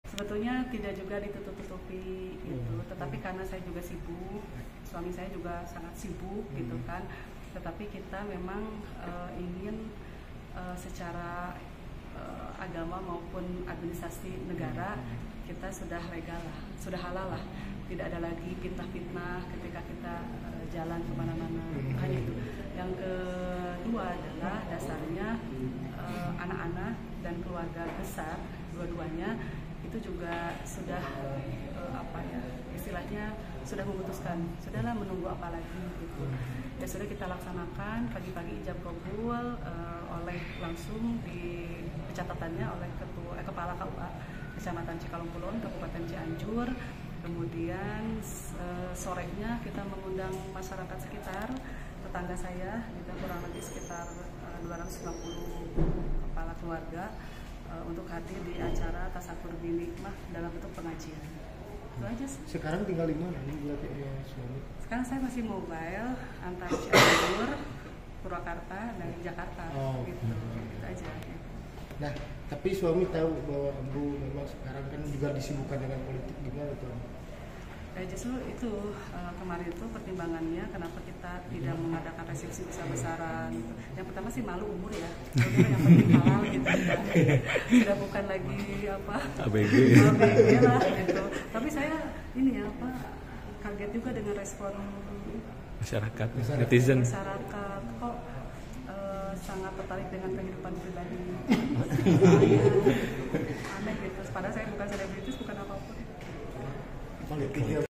TERBONGKAR, Mantan Bupati Purwakarta Anne Ratna Mustika Nikah Lagi, Masyarakat Kok Jadi Kepo ya
Laporan Wartawan Tribunjabar.id Nazmi Abdurrahman

TRIBUNJABAR.ID, BANDUNG - Pernikahan mantan bupati Purwakarta Anne Ratna Mustika dengan Iskandar sempat jadi sorotan publik.

Pernikahan Anne pada Sabtu 16 Desember 2023 itu digelar secara tertutup di kediamannya di Kabupaten Cianjur.

"Kenapa kita tidak mengadakan resepsi besar-besaran? Yang pertama sih malu ya, umur. Ya, sudahlah, yang penting halal," ujar Anne saat ditemui di Pengadilan Negeri (PN) Bandung, Rabu (3/1/2024).

Editor Hermawan | Ve Oki Priyana